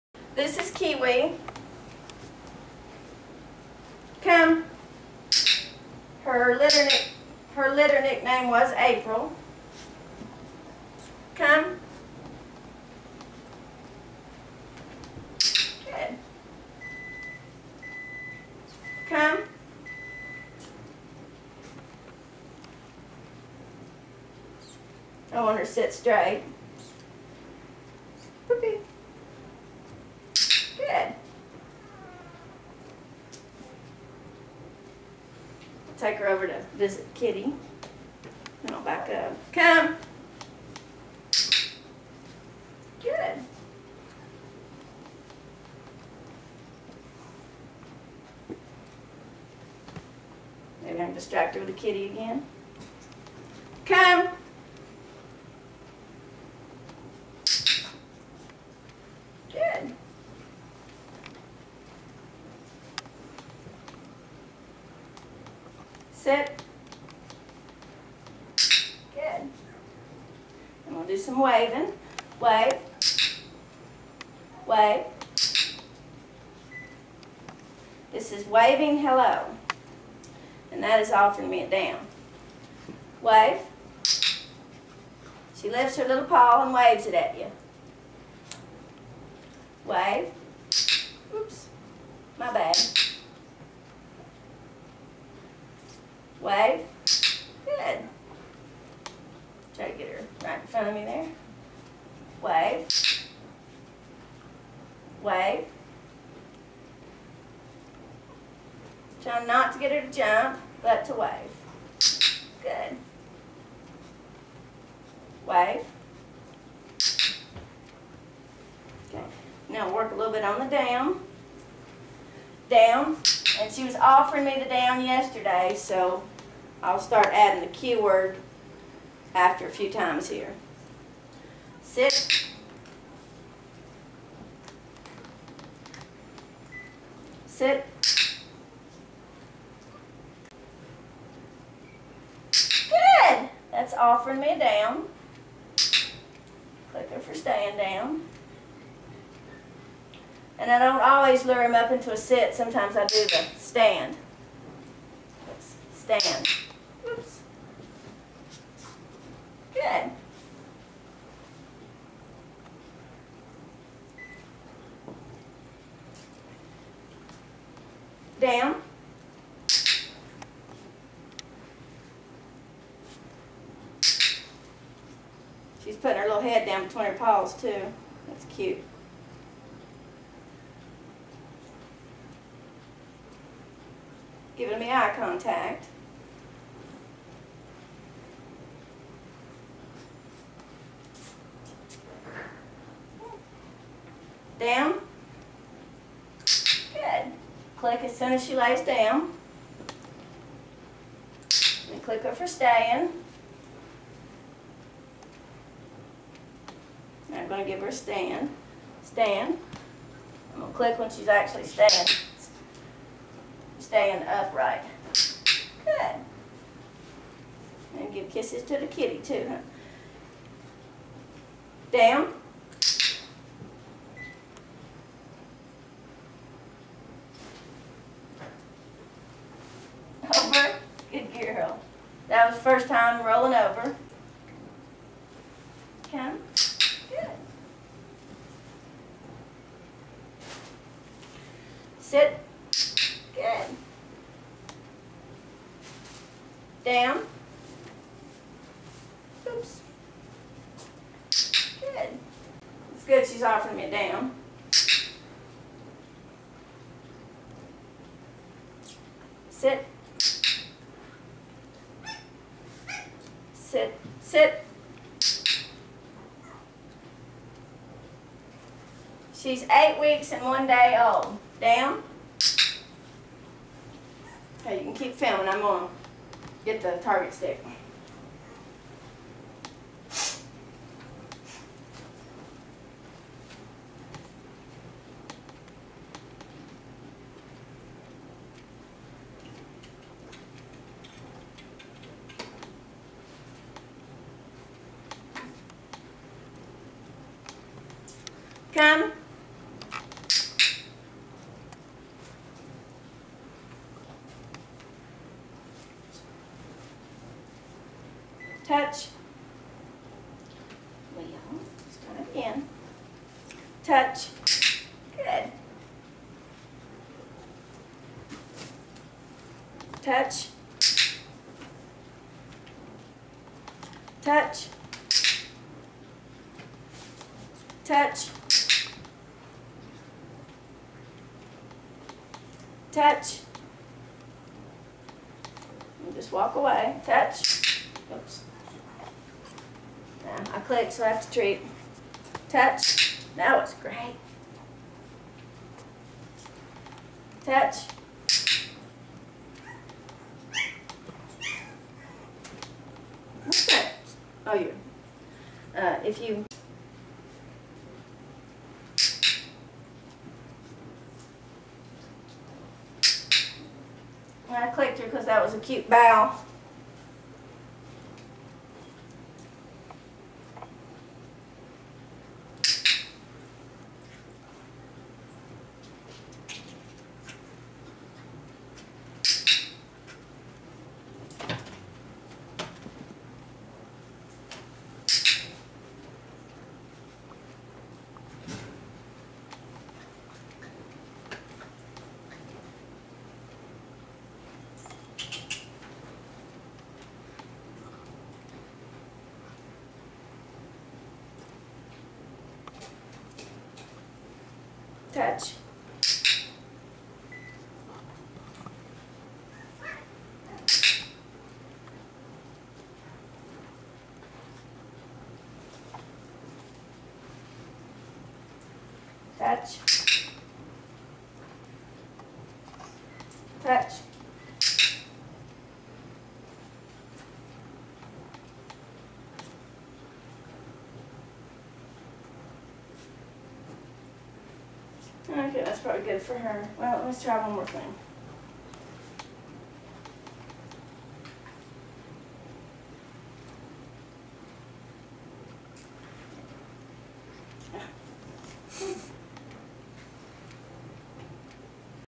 Kiwi clicker training at 8wks
Kiwi (was April) at 8 weeks old being clicker trained.
She is a Lock-Eye Feena x Blastoff Lock-Eye Reece daughter.
There are 2 great pups still available in the litter.
June, a high drive black and white female and July a medium drive red and white male.